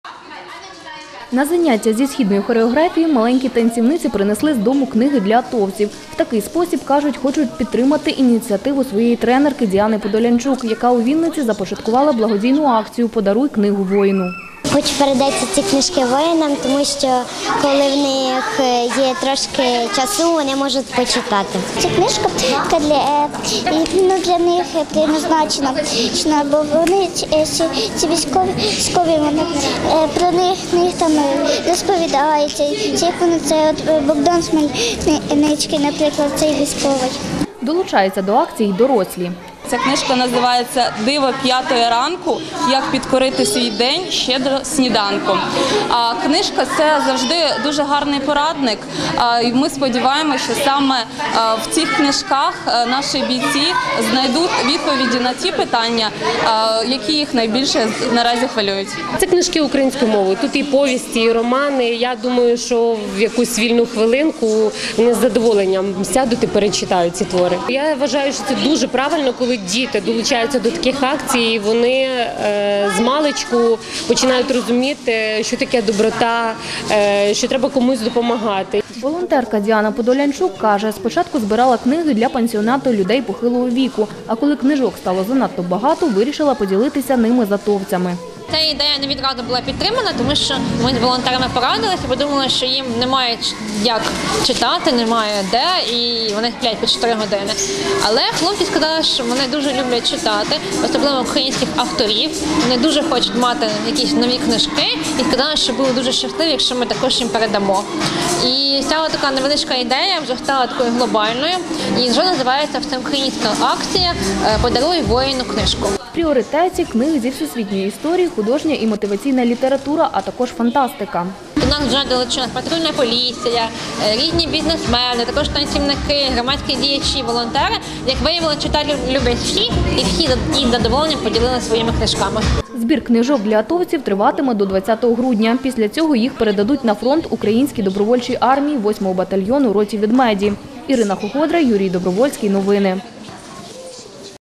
Акція “Подаруй книгу воїну”
Автор: Ірина Хоходра.

У Вінниці триває акція «Подаруй книгу воїну». Її ініціювала волонтерка Діана Подолянчук. Каже, спершу літературу збирали для людей поважного віку, які перебувають у пансіонаті геріатричного профілю, а коли книжок стало за багато, вирішили поділитися ними з атовцями. У пріоритеті книги зі всесвітньої історії, художня і мотиваційна література, а також фантастика. Книги передадуть бійцям роти «Ведмеді», яка входить до складу 8 батальйону Української Добровольчої Армії.